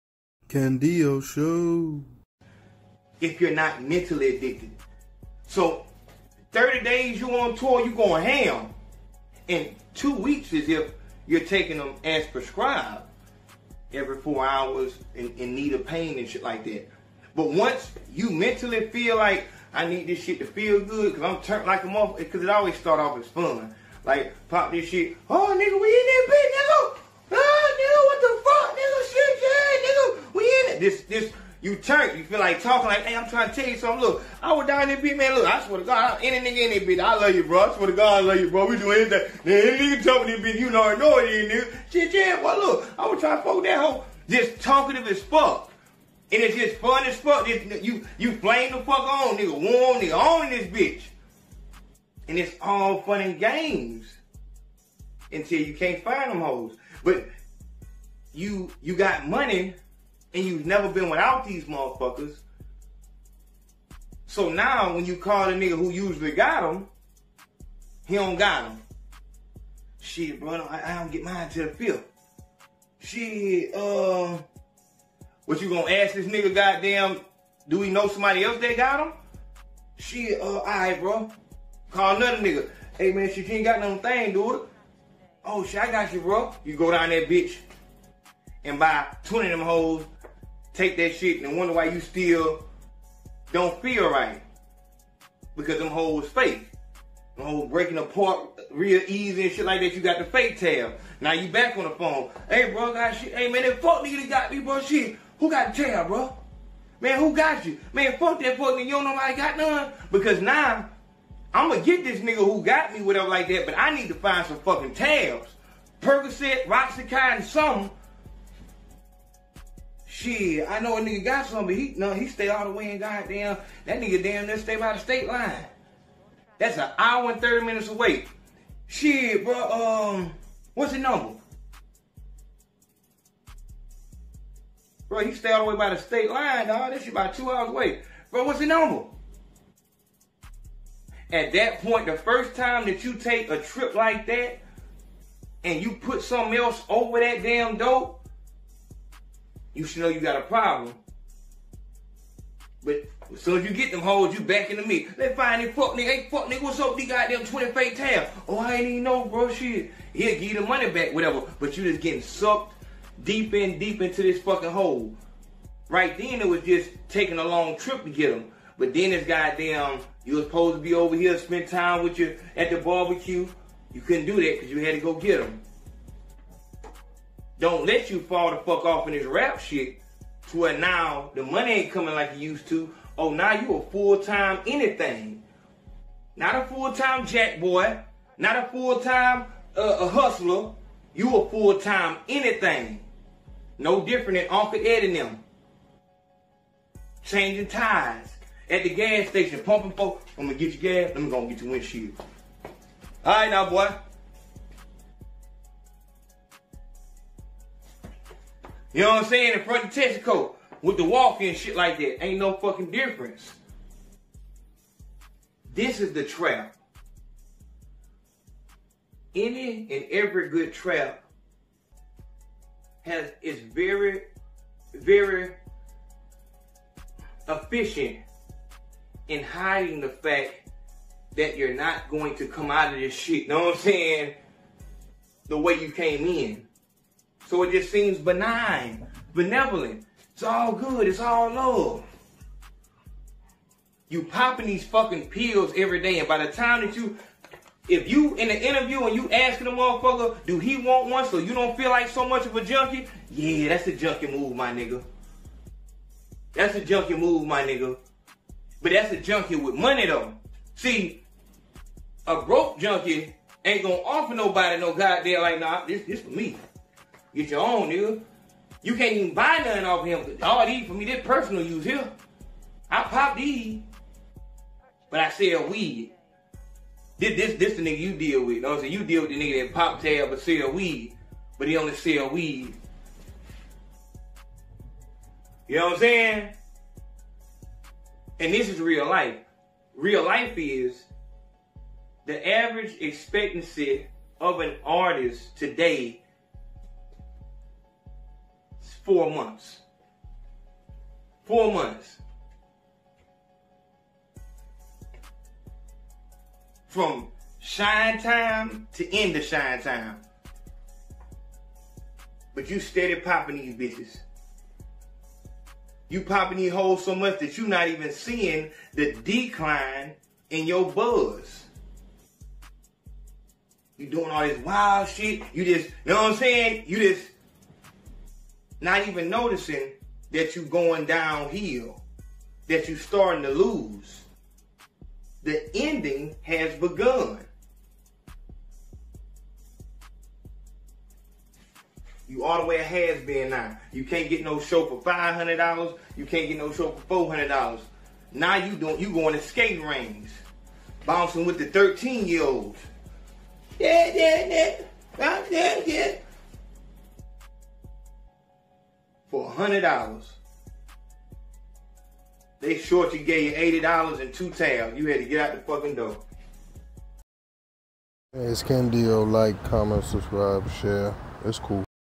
The "PlLL" Rap Trap Cartoon | How Labels get Rappers H00KED
the rap labels had to adjust and on The Rap Trap our host AYO CONSACO breaks down the lyrics to see if they match what happened in real life.